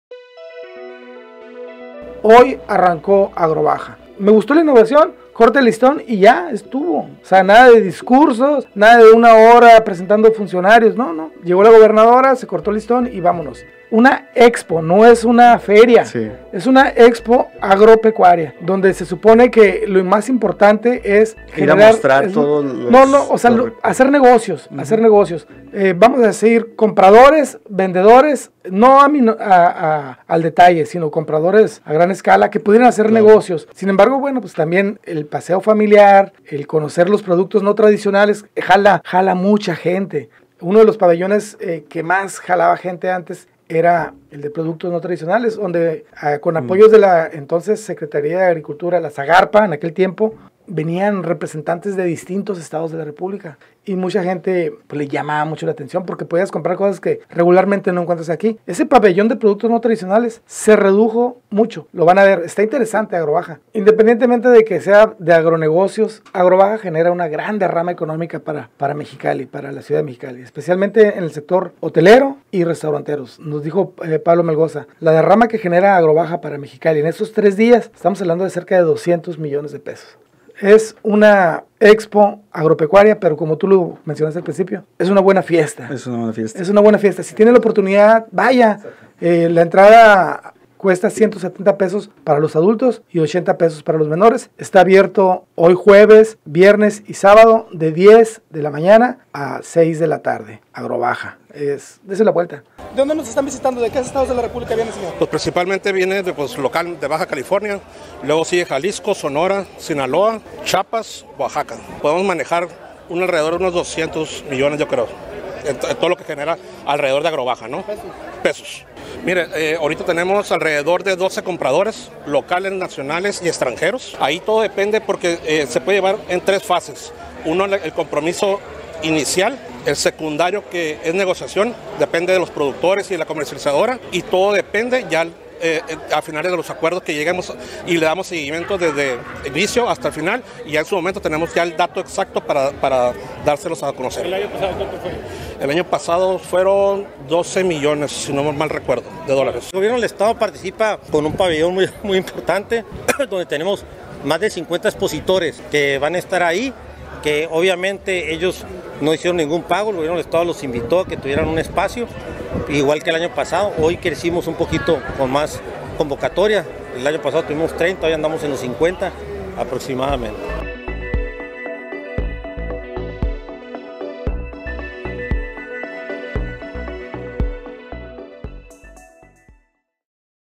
Zero Noticias - Inició Edición 2023 de AgroBaja
La expo agropecuaria Agrobaja, que se realiza en las inmediaciones del Campo Experimental del INIFAP, abrió sus puertas esta jueves 2 de marzo con más de 500 expositores, explicó el Presidente del Patronato, Pablo Melgoza.
Dijo que para esta edición esperan alrededor de 50 mil visitantes una derrama económica de alrededor de 200 millones de pesos para Mexicali y que se concreten negocios por varios millones de dólares.

Sintoniza Zero Noticias de Lunes a Viernes a partir de las 3:00 pm en YouTube o Facebook